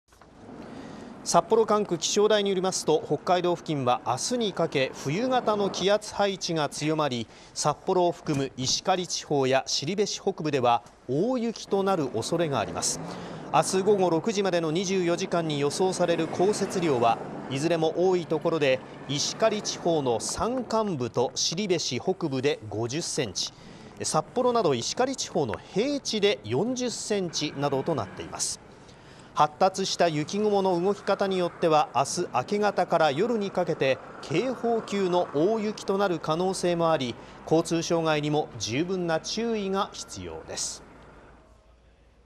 【ドカ雪の恐れ】札幌など石狩地方の平地で最大４０センチ 通勤にも影響か
あすの明け方から夜にかけて、札幌を含む石狩地方や後志北部で警報級の大雪となるおそれがあります。交通障害などに十分な注意が必要です。